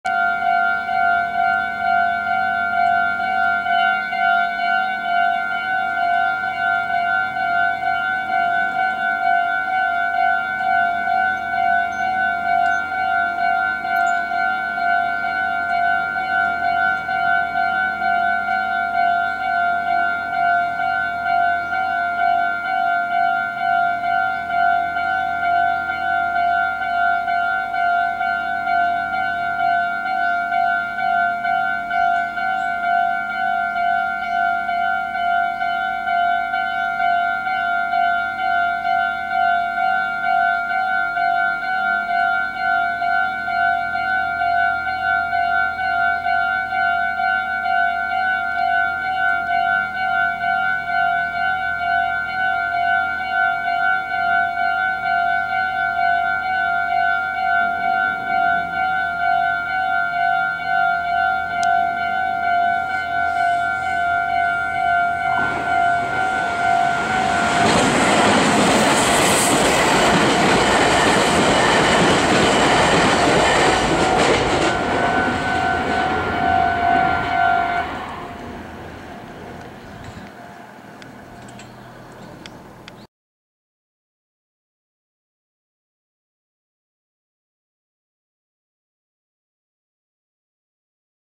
名鉄踏切名古屋本線 岐南5号
登場列車
新型3100系+3300系（3104F+3313F)急行岐阜行き　列車番号1213